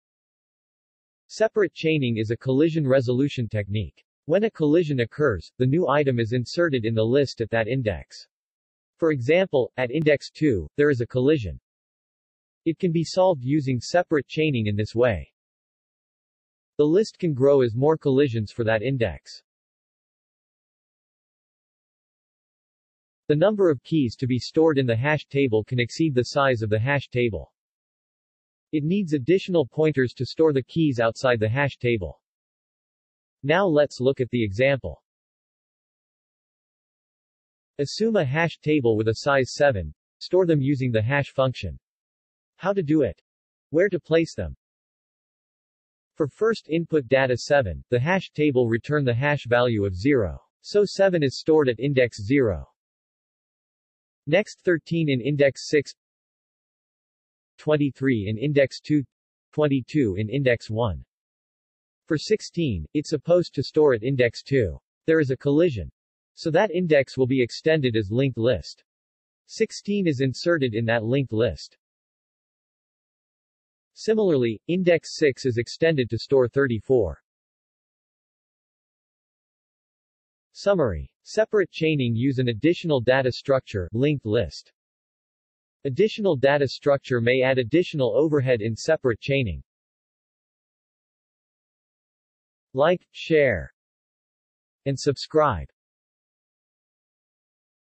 Separate Chaining explained in 1 minute -Part 5 Hashing in Data Structure
What is separate chaining? How to use separate chaining technique to solve hash collision? Find out the answers and separate chaining examples in this video in 1-minute.

It's a collection of one minute maths refresher videos for those who want to learn data science, machine learning and statistics.